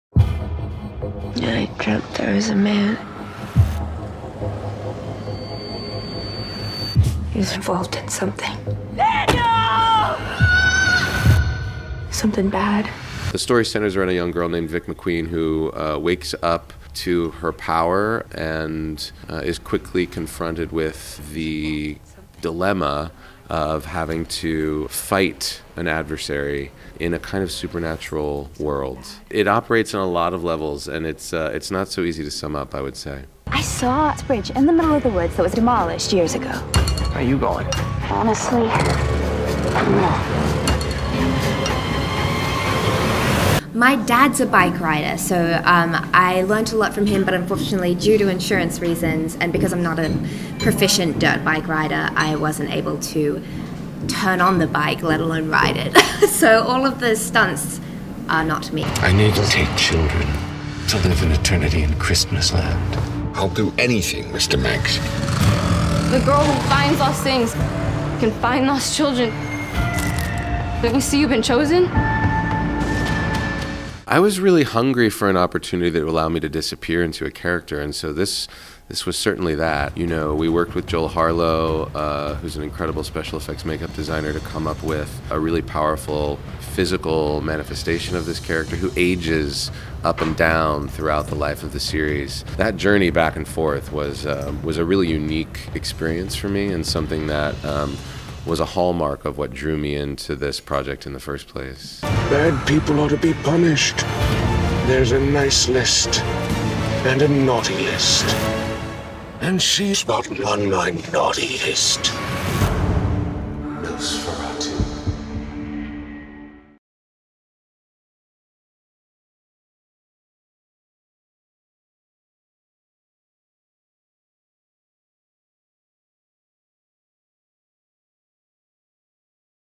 Quinto, Cummings on NOS4A2
Welcome to Christmas Land! AMC's upcoming supernatural horror series NOS4A2 will take audiences to a cold and twisted Christmas village ruled by a dangerous man: Charlie Manx. Based on the novel of the same name by Joe Hill (aka Joseph Hillstrom King, son of Stephen King), the story follows small-town, teenage high school senior Victoria "Vic" McQueen as she discovers that she has a supernatural ability to track Charlie, a man who feeds off the souls of children. The series introduces the talented Australian actress Ashleigh Cummings who plays the fearless motorcycle-riding Victoria. Transformed by facial special effects, actor Zachary Quinto disappears in his character as Manx. HFPA members Mirai Konishi, Jenny Cooney and Anke Hofmann had a chat with the cast before the show premieres on AMC.